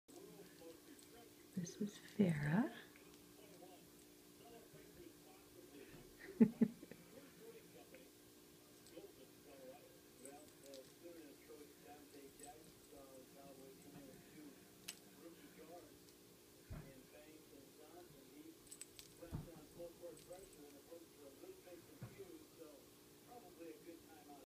Video of adoptable pet named Farah
Farah is an adoptable pet with Laramie Animal Welfare Society in Laramie WY.